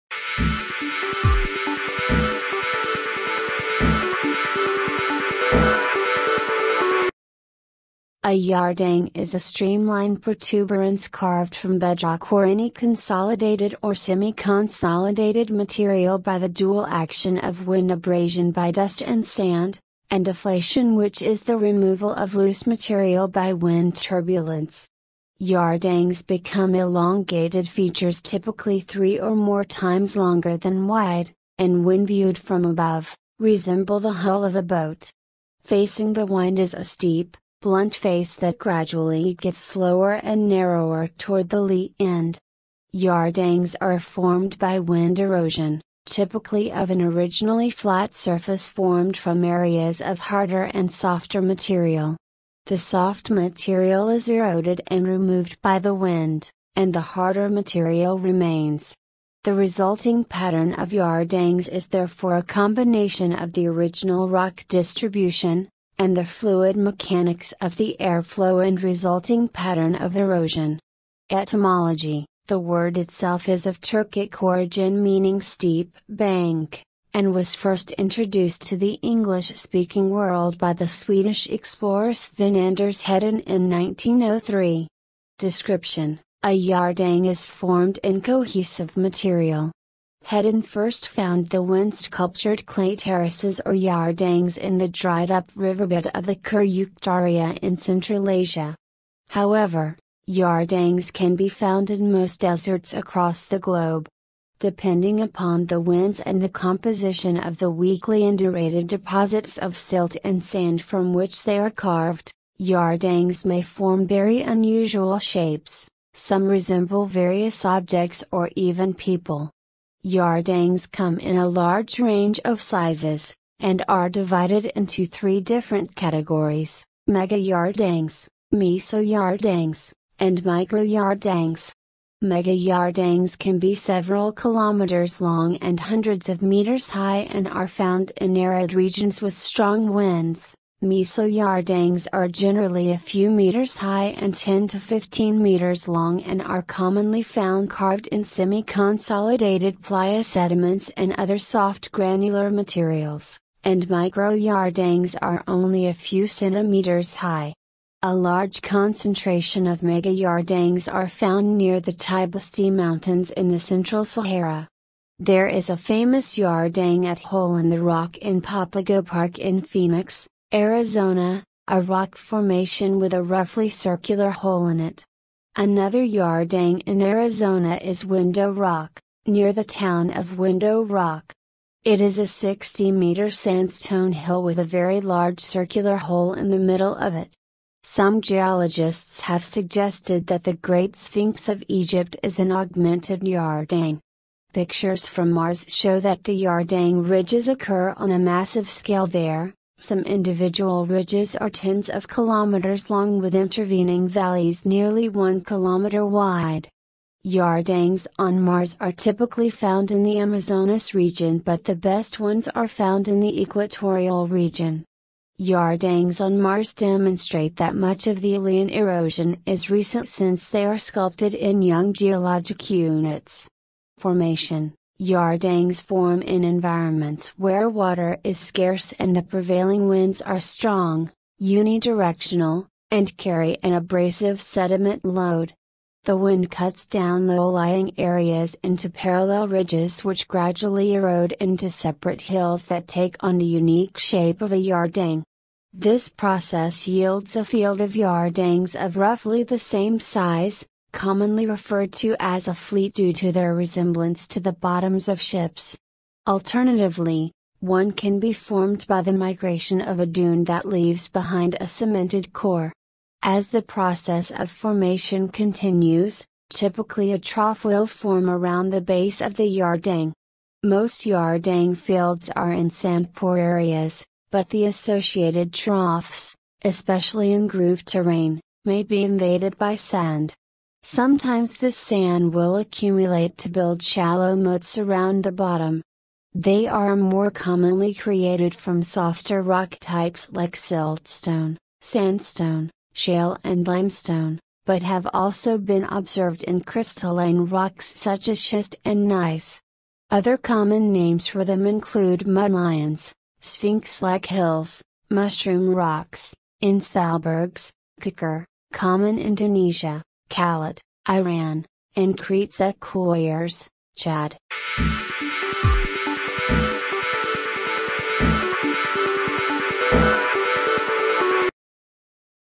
Yardang
A yardang is a streamlined protuberance carved from bedrock or any consolidated or semiconsolidated material by the dual action of wind abrasion by dust and sand, and deflation which is the removal of loose material by wind turbulence. Yardangs become elongated features typically three or more times longer than wide, and when viewed from above, resemble the hull of a boat. Facing the wind is a steep, blunt face that gradually gets lower and narrower toward the lee end. Yardangs are formed by wind erosion, typically of an originally flat surface formed from areas of harder and softer material. The soft material is eroded and removed by the wind, and the harder material remains. The resulting pattern of yardangs is therefore a combination of the original rock distribution, and the fluid mechanics of the air flow and resulting pattern of erosion.